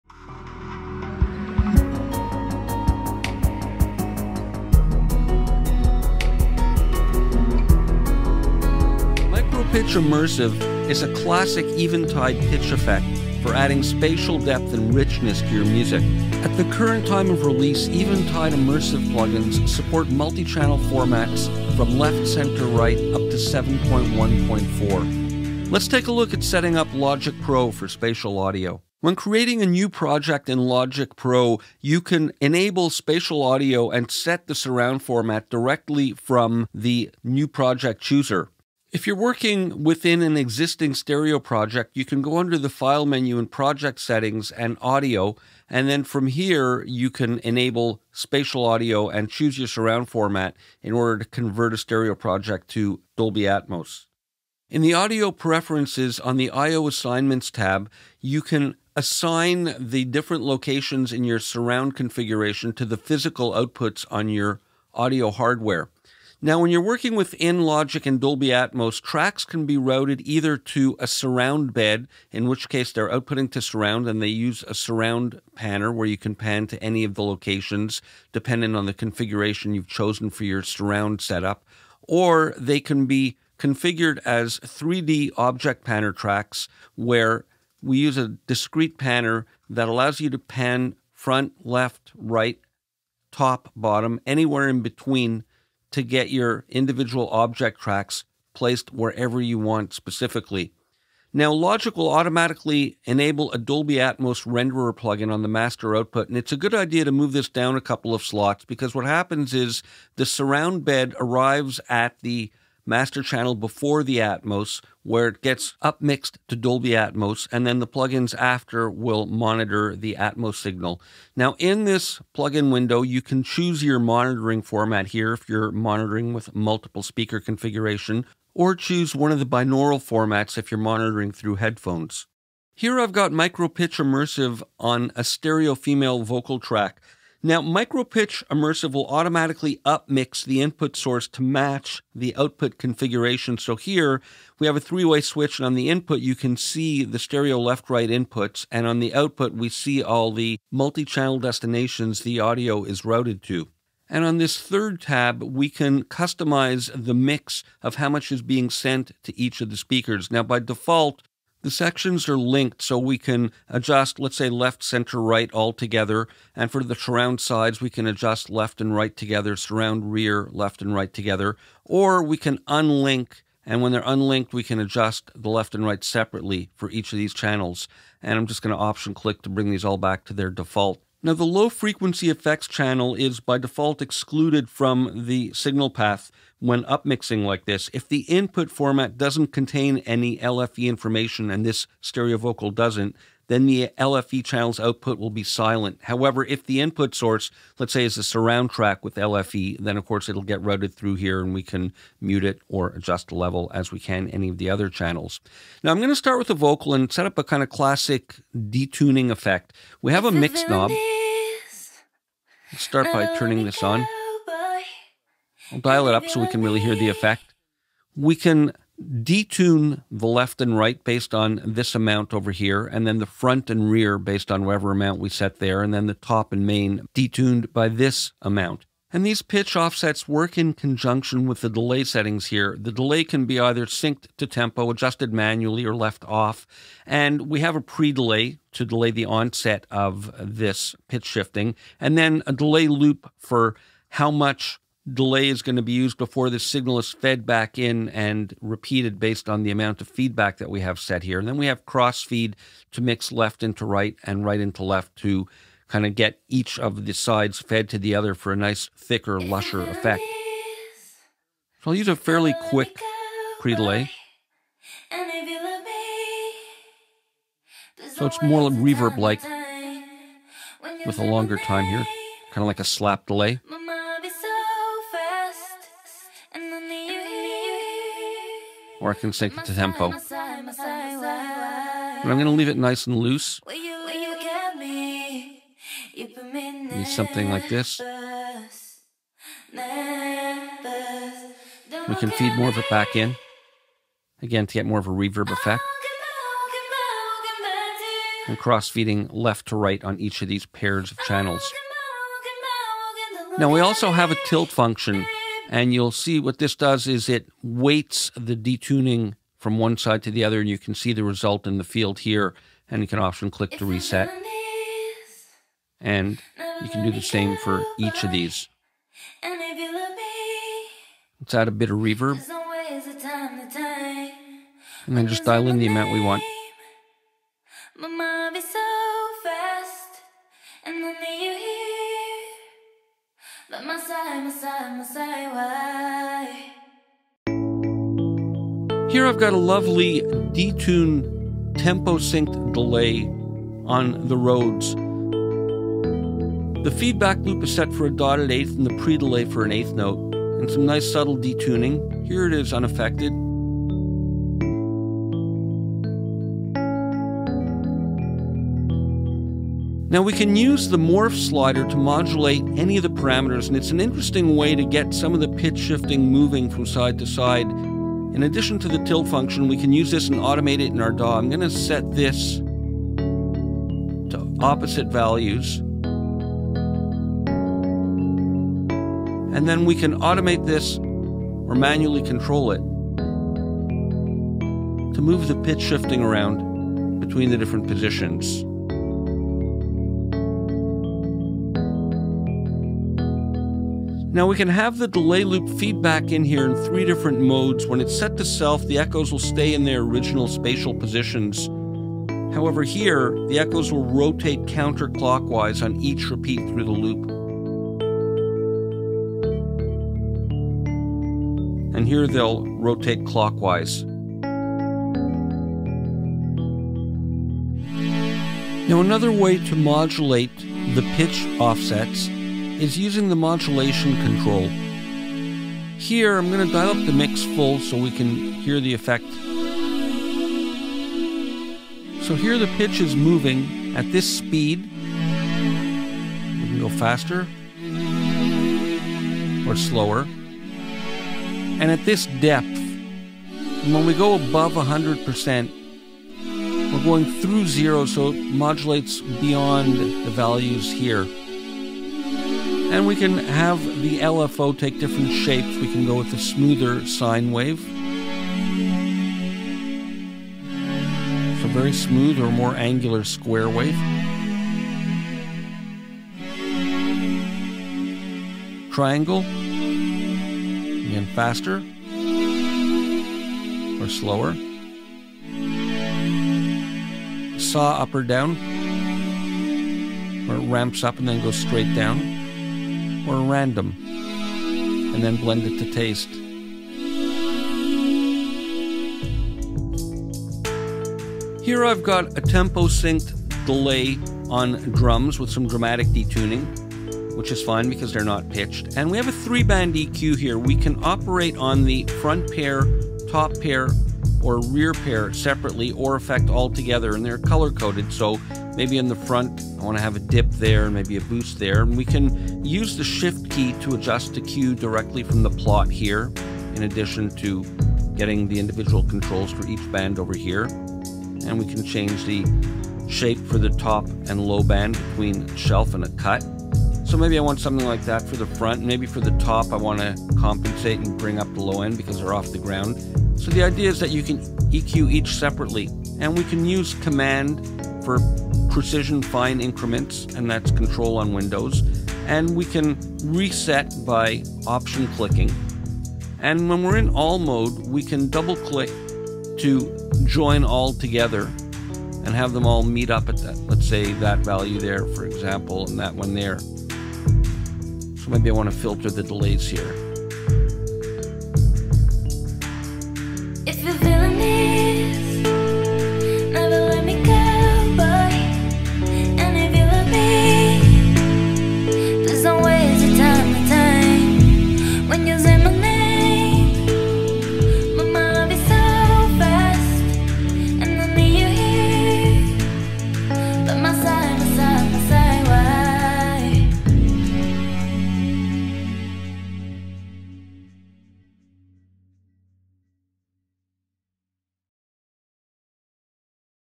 Eventide MicroPitch Immersive Plug-in: Overview & Demo
MicroPitch Immersive users can spread, tilt, and dynamically morph the effect across the width, depth, and height of an immersive mix.

With up to 12 channels of spatialized, detuned delays, plus modulation, EQ, and crossfeed controls, MicroPitch Immersive excels in creating both real and unreal immersive realities with boundaries that reflect, move, diffuse, and dissolve. Tilting the Left/Right, Front/Back, and Top/Main Detune parameters allow quick and easy asymmetrical detuning and space fine-tuning. The plug-in’s enhanced delay and modulation capability can be used to create slapbacks, add movement with pitch modulation, or create exciting motion with patterned Loop Delay.

Explore the MicroPitch Immersive Plug-in in this Overview by Eli Krantzberg.

0:00 - Intro
0:44 - Setting Up Spatial Audio in Logic Pro
2:26 - MicroPitch Controls & Customization
3:26 - Classic Detuning Effect Setup
5:42 - Creative Use on Instruments
6:28 - Modulating Pitch with Morph Slider
8:24 - EQ Customization and Final Touches